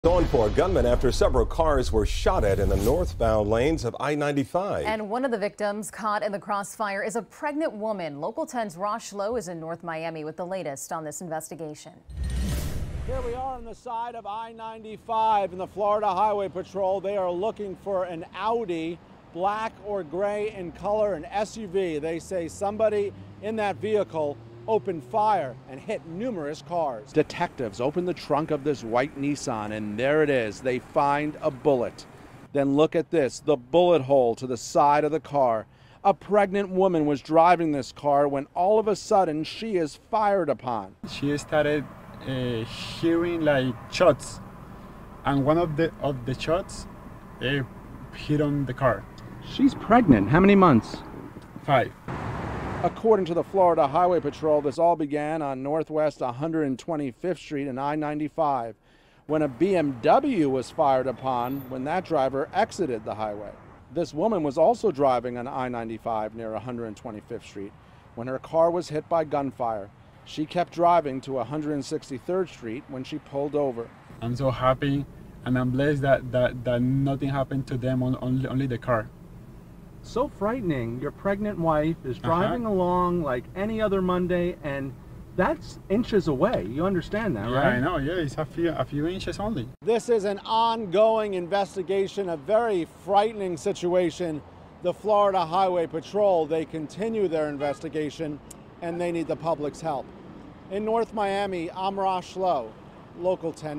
Detectives search for I-95 gunman in North Miami
Detectives were asking the public for help with identifying the gunman involved in a shooting on Monday on Interstate 95 in North Miami.